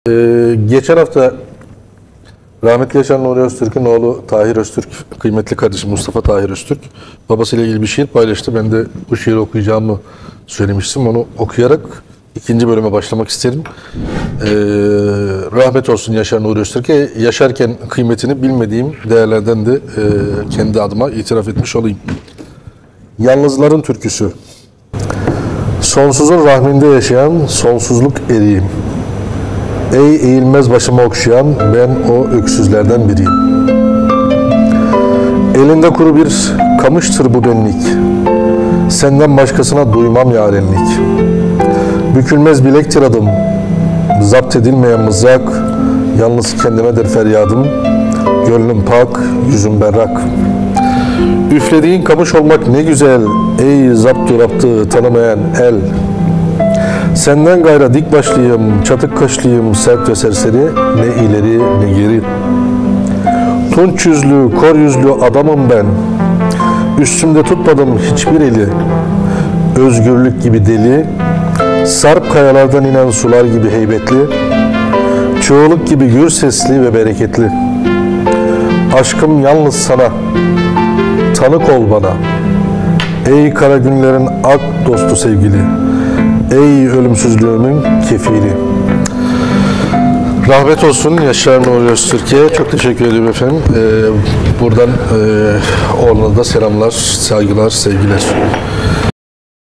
Yaşar Nuri Öztürk'ün "Yeni Kitabı"nın Habercisi Şiiri: Yalnızların Türküsü
Prof. Dr. YaşarNuriÖztürk 'ün "Herkesle ve Hiçkimsesiz" kitabının habercisi niteliğindeki "Yalnızların Türküsü" şiirini, Flaş Tv. kanalından Veysi Dündar bey okudu. Her ikisine de teşekkür ederiz...

BAĞLANTILAR:

▶ KİTAP:
Yaşar Nuri Öztürk Kitapları "Adrese Teslim" Kolaylığıyla